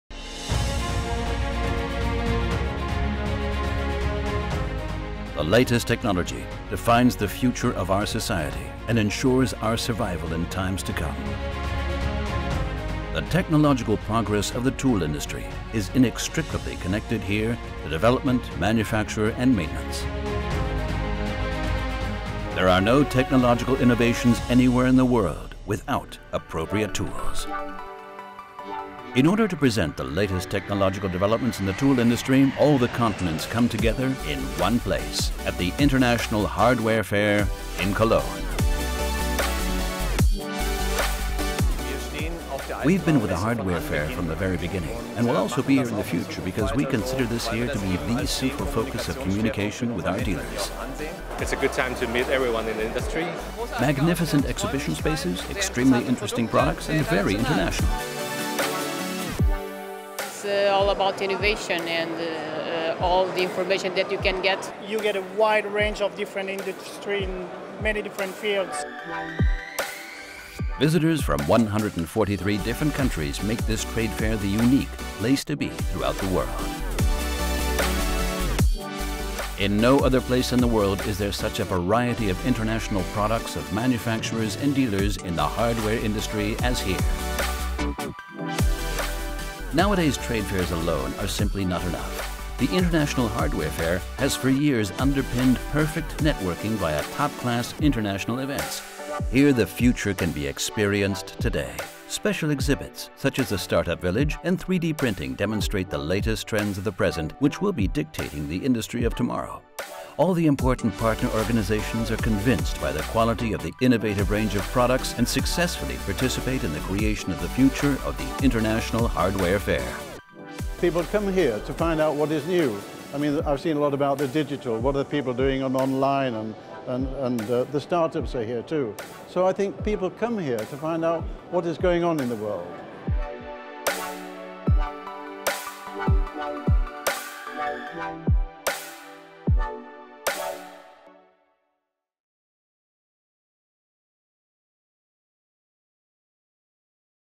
EISENWARENMESSE - Image film - English
We present the image film of EISENWARENMESSE.

The INTERNATIONAL HARDWARE FAIR 2020 takes place on March 1-4 2020 in Cologne.